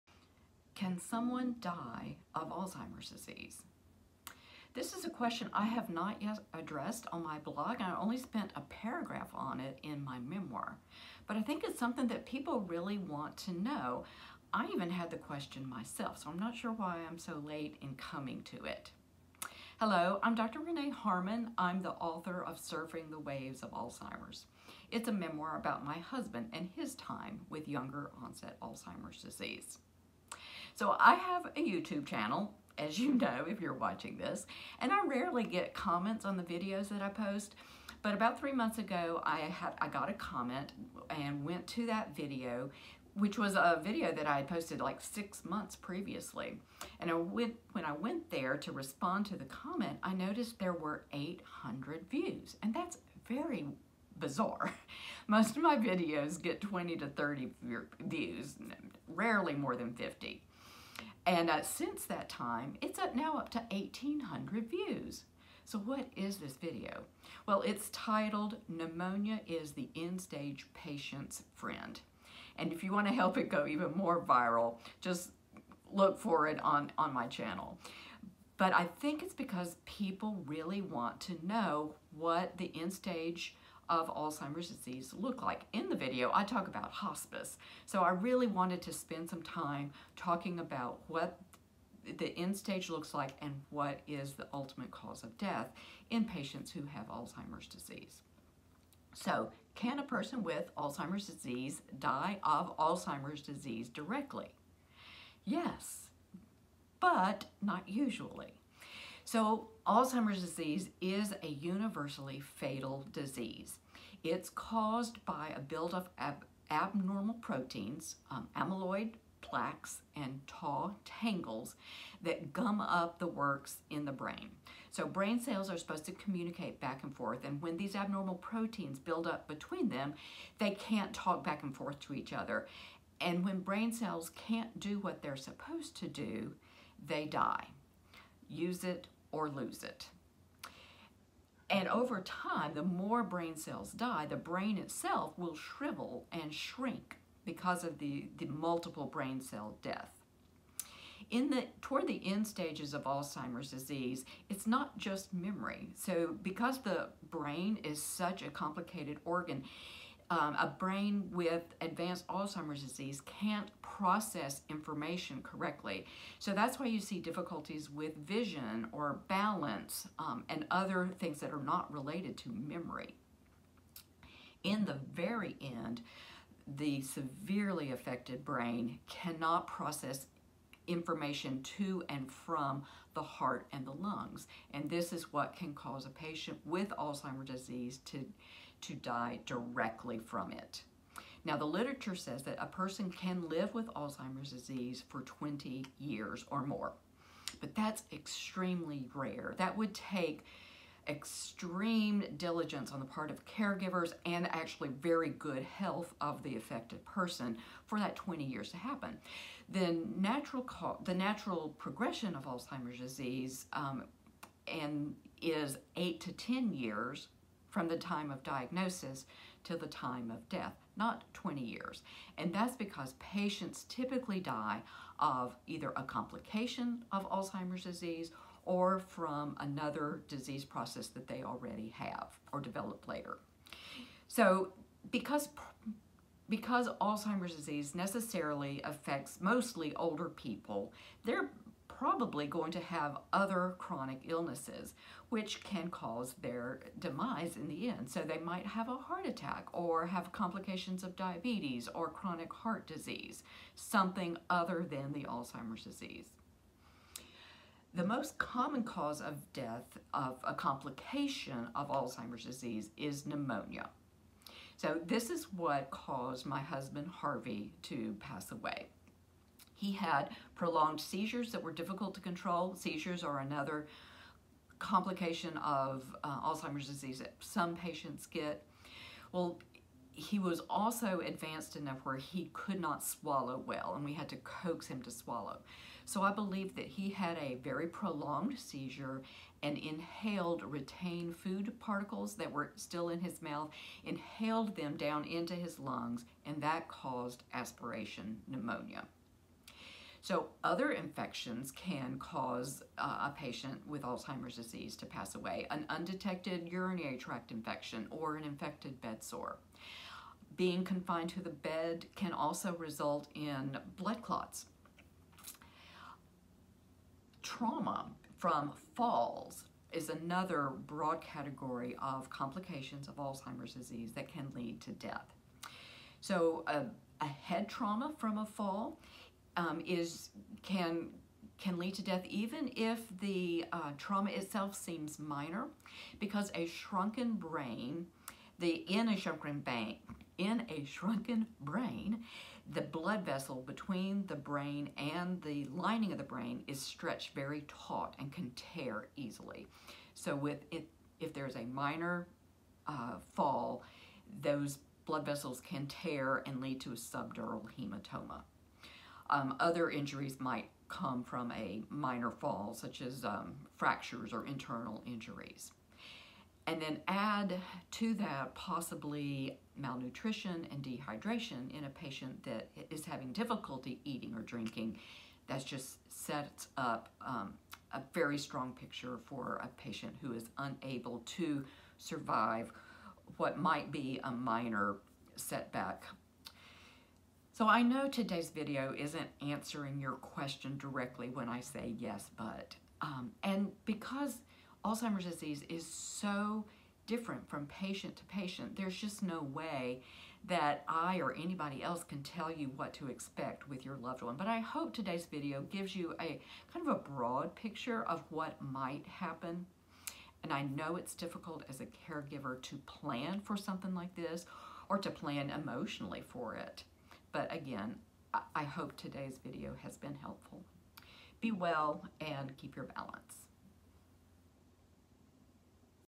Can Someone Die of Alzheimer’s Disease?
The answer is, “Yes, but…” In today’s video, I discuss how a person affected by Alzheimer’s disease may succumb to the disease itself, from another disease process, or from a complication of their Alzheimer’s.